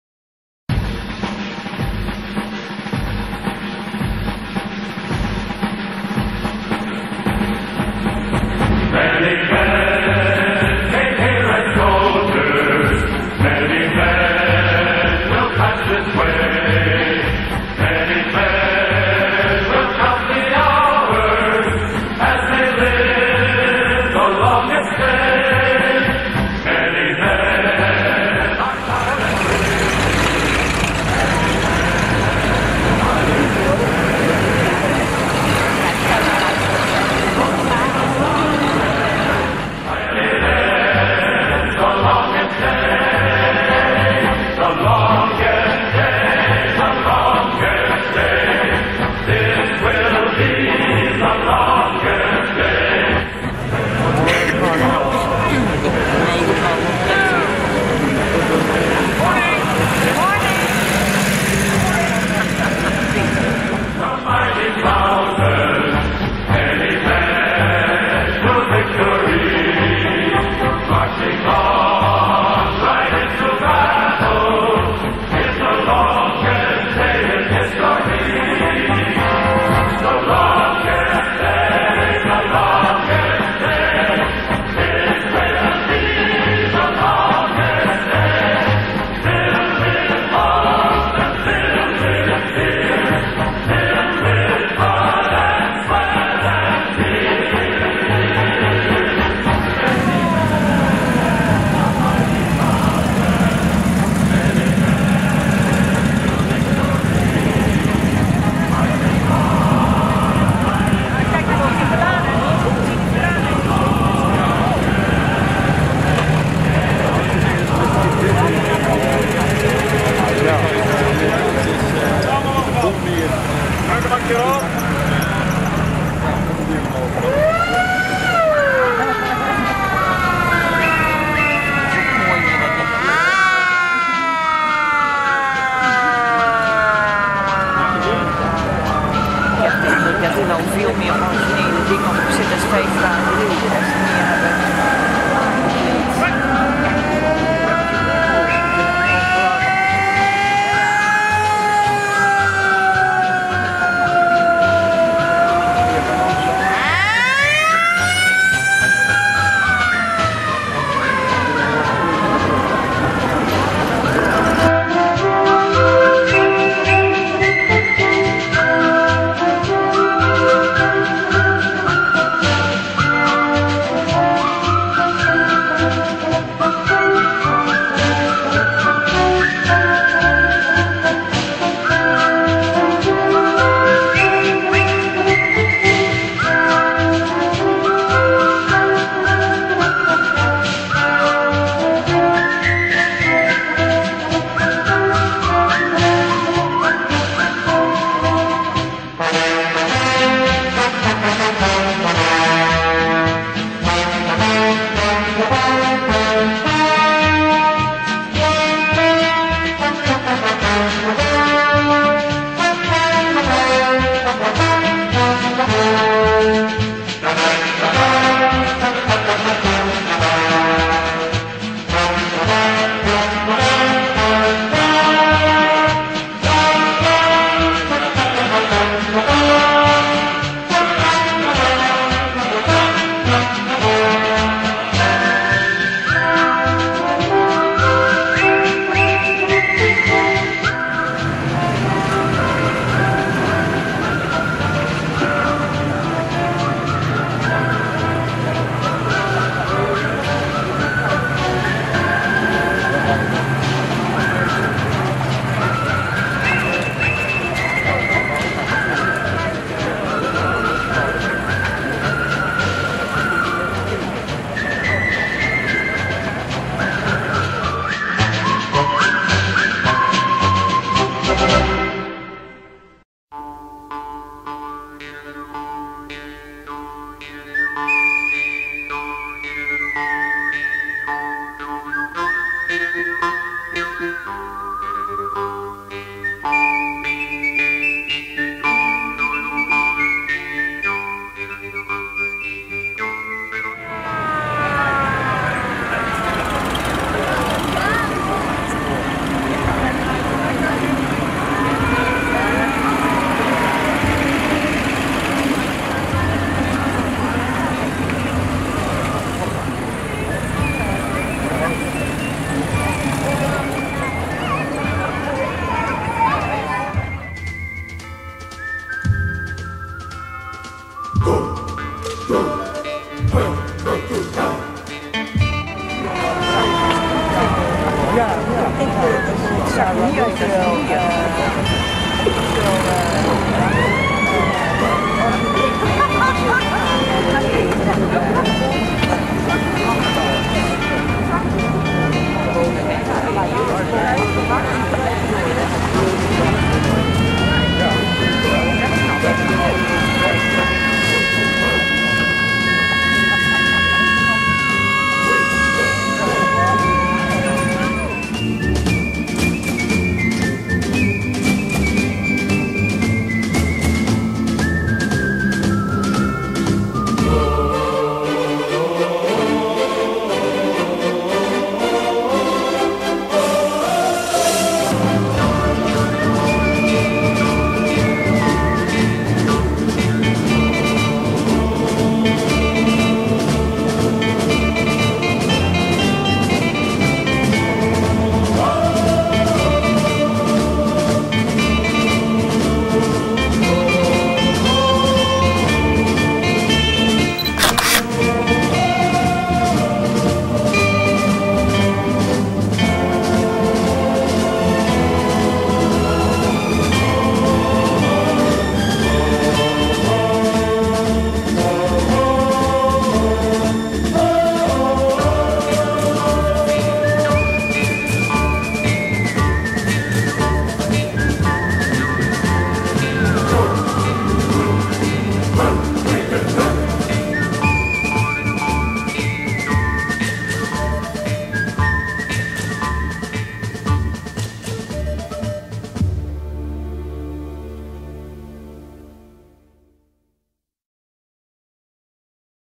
Operatie Market Garden - route Noord (22 september 2019)
Zondagmorgen 22 september 2019 trokken ruim 400 historische legervoertuigen van Veghel, Uden, Grave, Mook, Groesbeek naar Nijmegen. Dit in het kader van de 75-jarige herdenking van de bevrijding van Nederland.